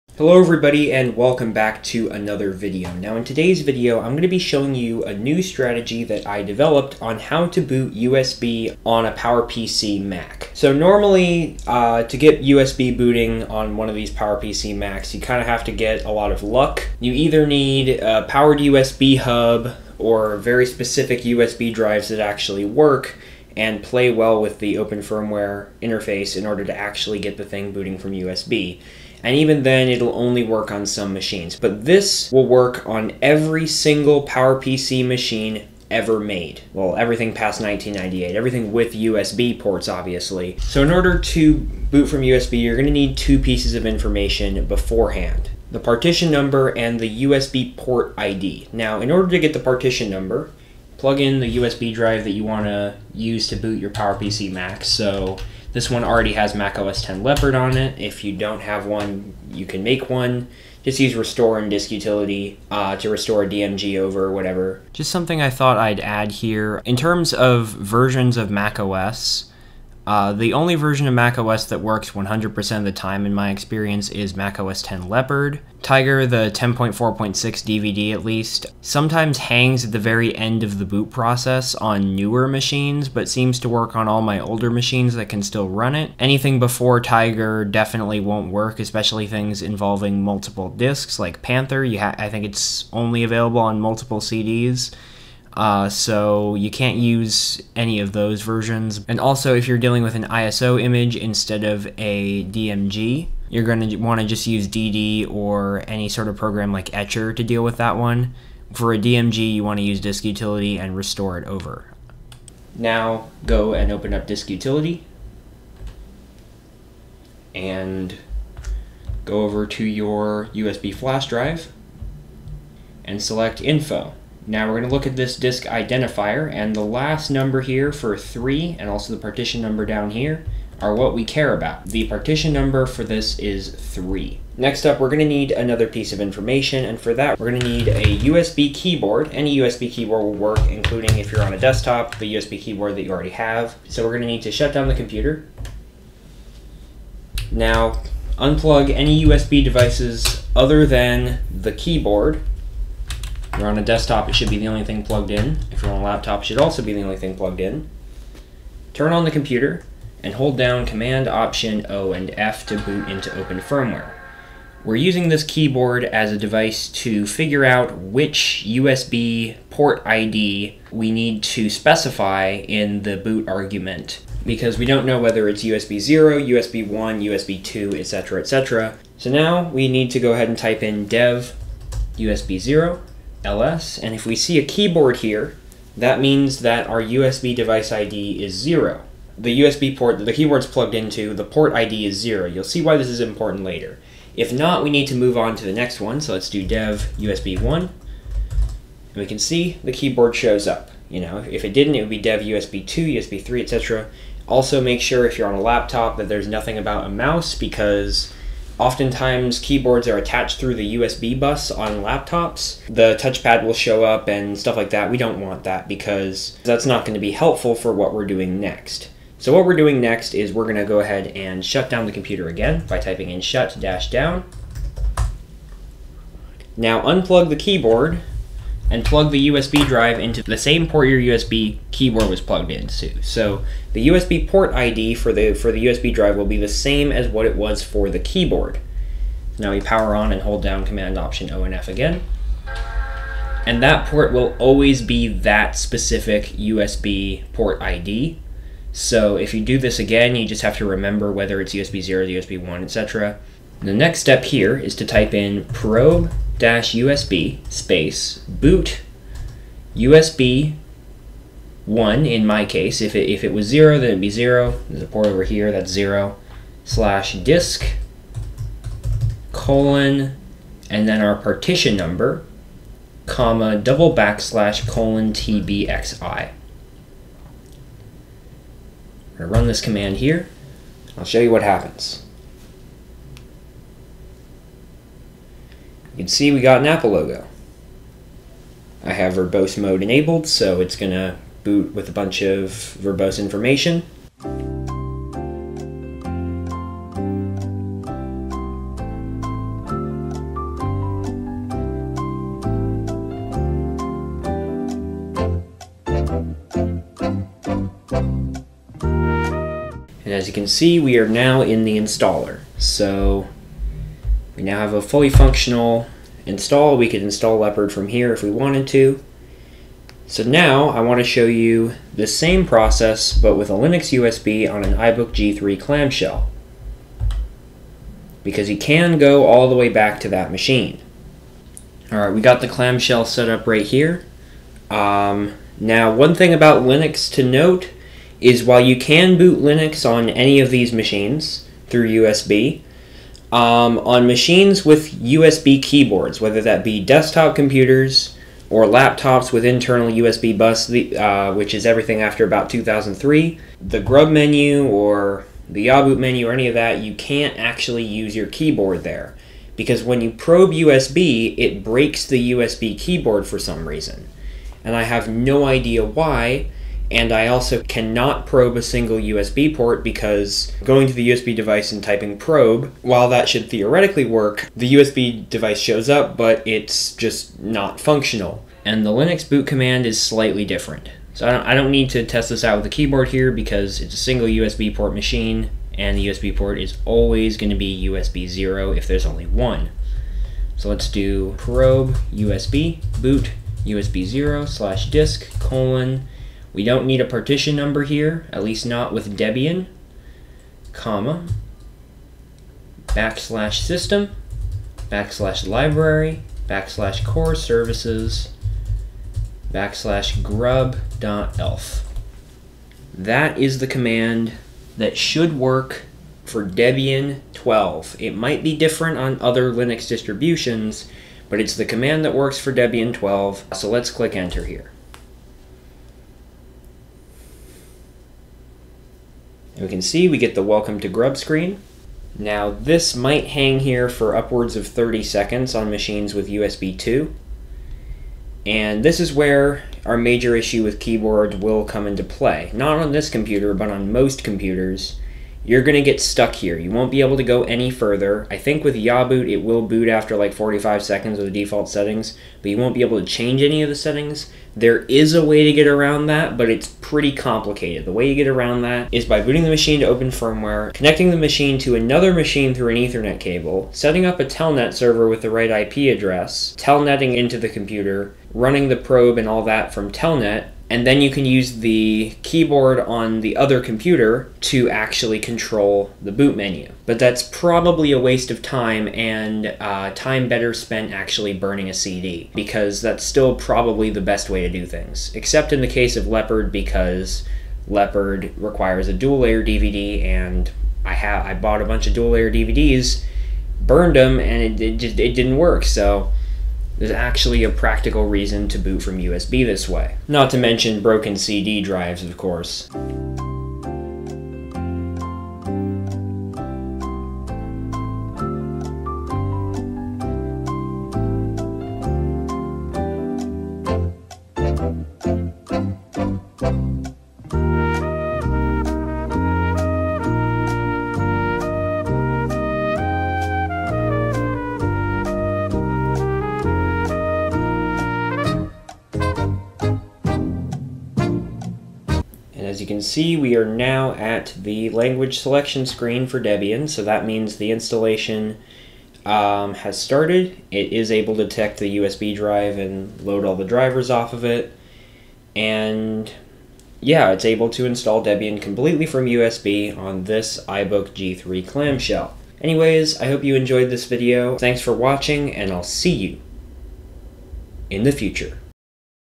NEW METHOD: How to boot from USB on Most NewWorld PowerPC Macs!
It’s worth noting that I changed the title from “ANY PowerPC Mac” to “Most NewWorld PowerPC Macs” because when I made this video, I thought this worked on all PowerPC Macs because it worked on all the ones I had, spanning most of the PowerPC era. However, I have since found out that this does not appear to work on certain PowerPC Macs, specifically from around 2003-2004.

Hello everyone, and welcome back to a new video! In today's video, I am going to show you a brand new method I found for booting from USB on PowerPC Macs. This method does not rely on getting lucky with USB drives or a powered USB hub and should work on every single PowerPC Mac with every single USB drive. The only limitation to this is whether your OS can boot from USB.

Socials:
Second Channel: @DistrosExtras